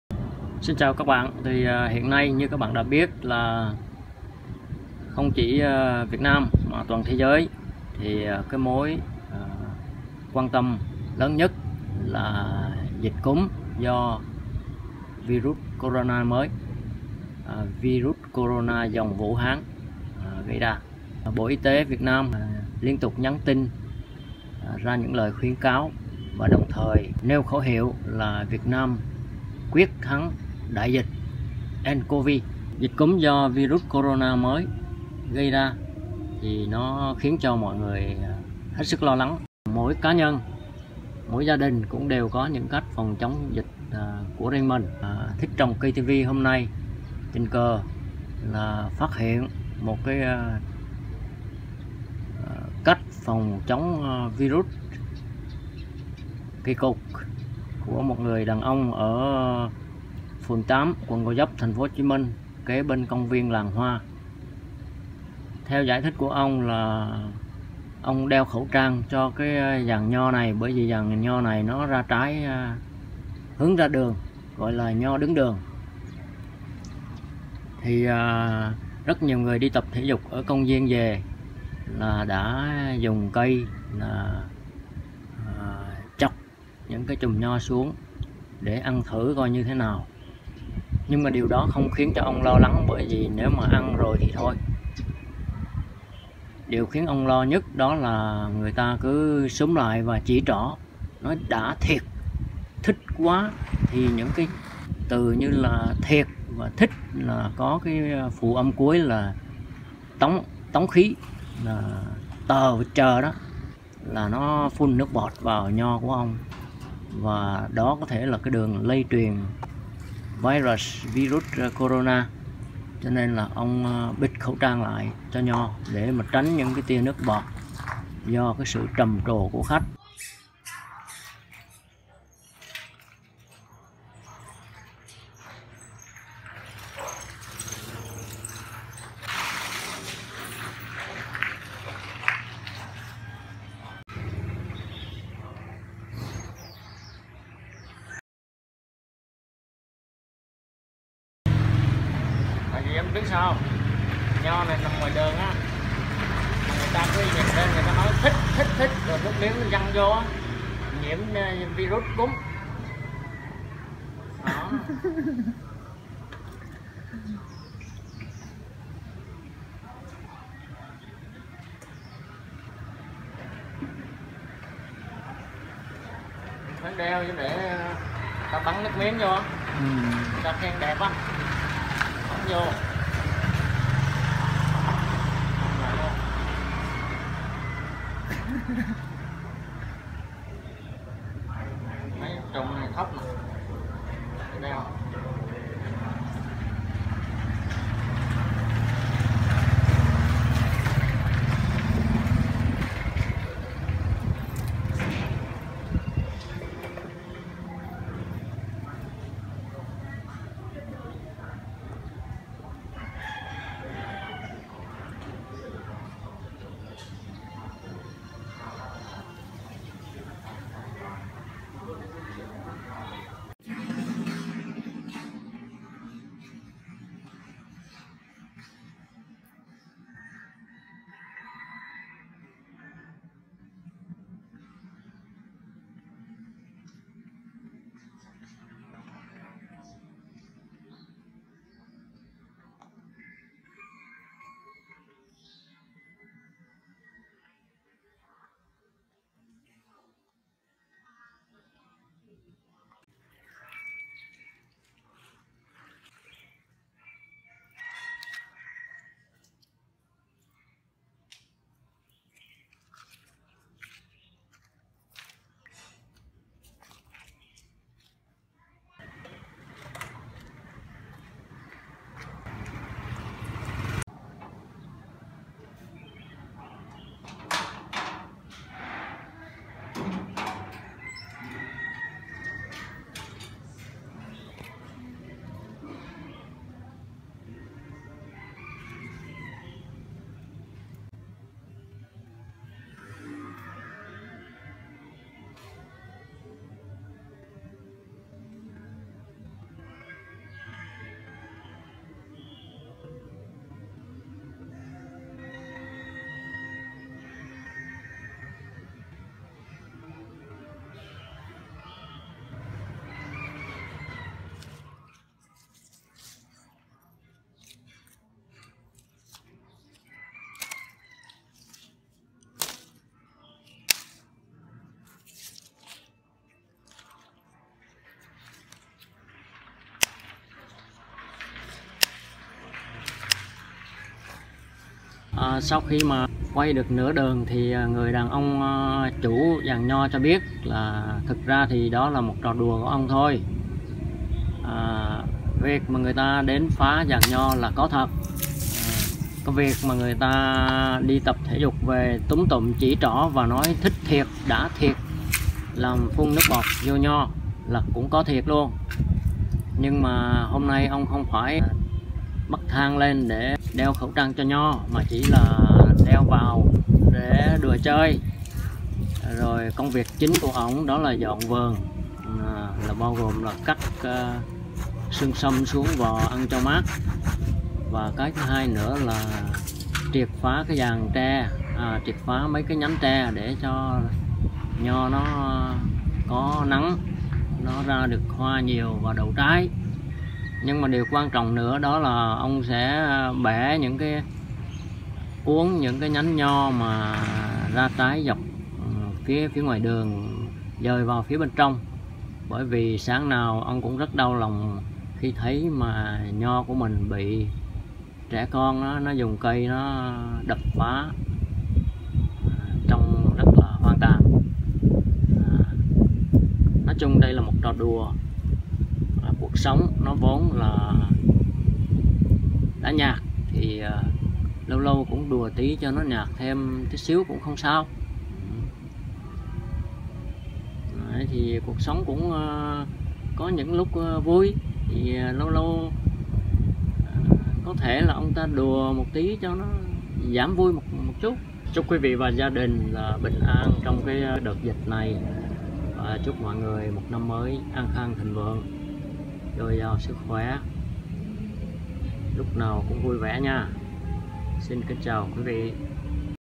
Người đàn ông đeo khẩu trang cho nho phòng dịch
Nho ra trái trước cổng, nhiều người đi tập thể dục ngang qua cứ xúm lại nói “thích thật”, bắn nước bọt lên nho, chủ giàn nho phải đeo khẩu trang cho nho để ngăn con di rúc theo nước bọt bắn vào.